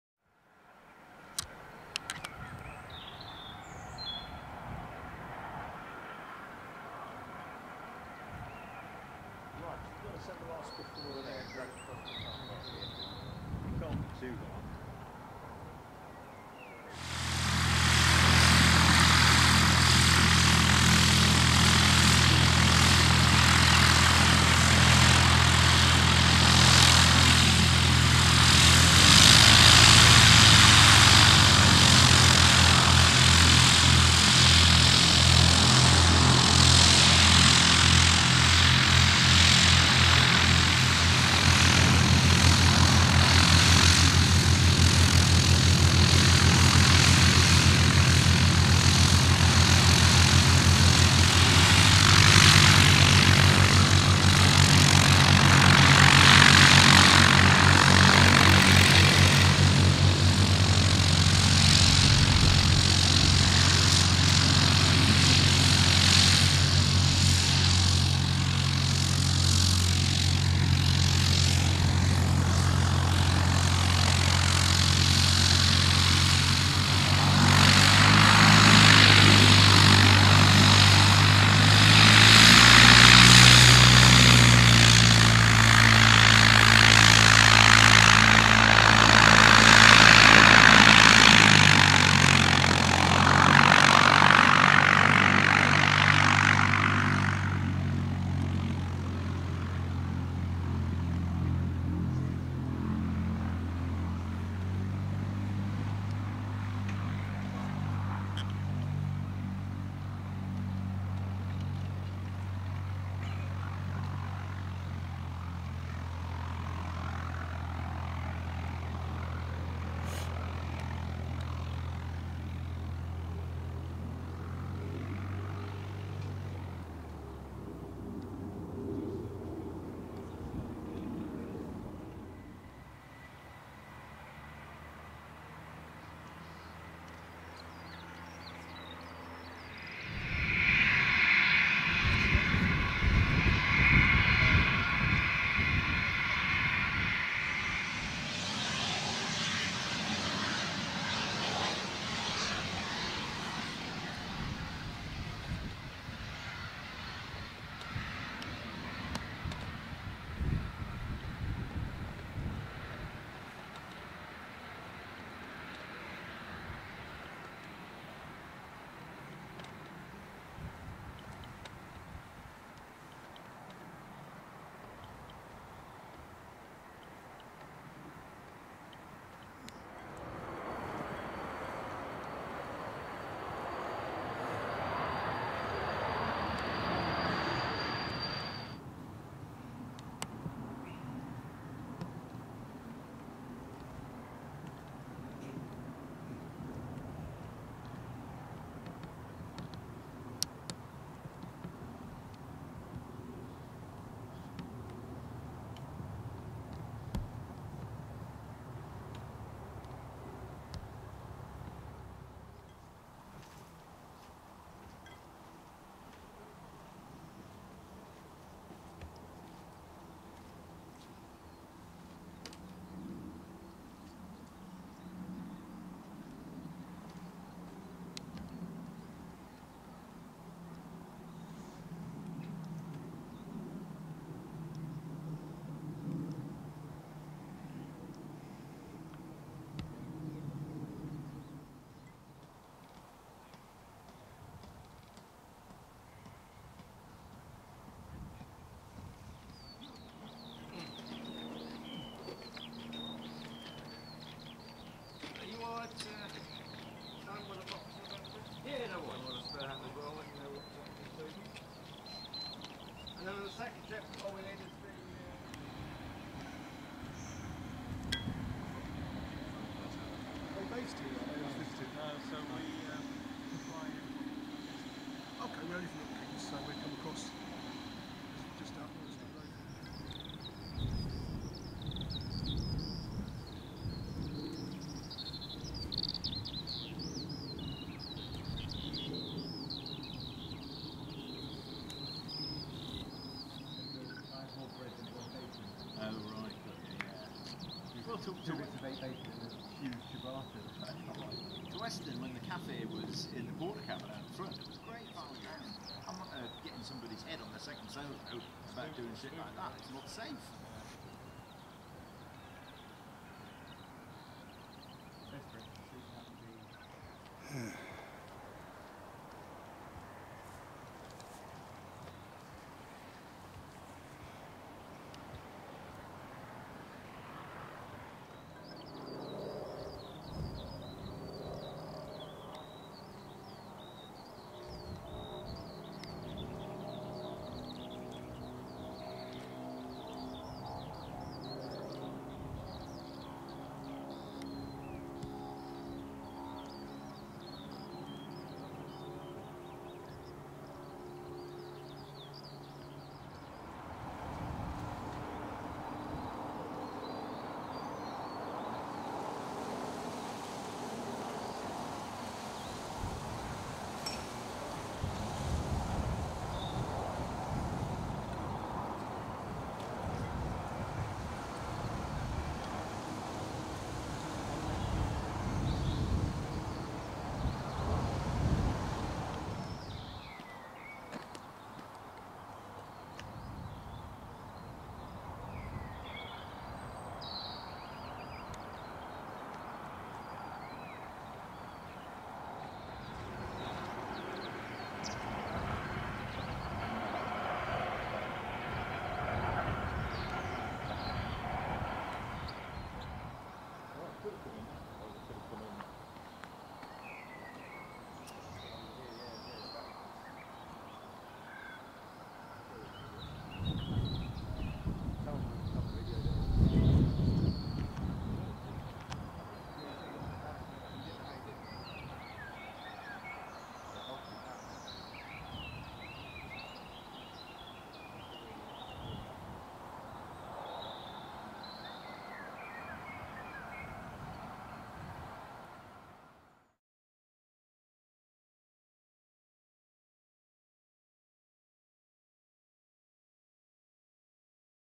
VISIT TO THE COTSWOLD AIRFIELD KEMBLE IN CIRENCESTER 27/05/22
This is a short video of when a very kind friend of mine took me on an afternoon visit to see the Cotswold Airfield at Kemble in Cirencester, Gloucestershire on 27th May 2022, it used to be an RAF base and a former home of the Red Arrows.